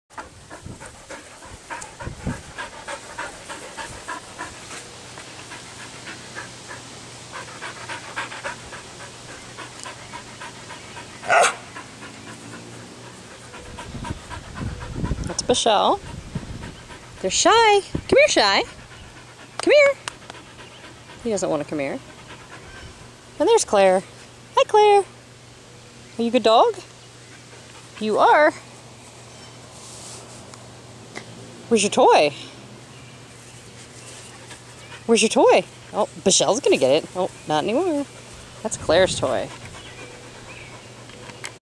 Belgian Sheepdog: Claire is almost 5-years-old
Claire's official name is:  Clarisse Dusoleil, CD, RE, PT, NAJ, JHDs, CGC.  She currently has 1 leg each toward 3 other titles:  CDX. HSAs, and NA.  We hope to earn those titles this year, plus her HRD1s and TD, and move into Open agility, too.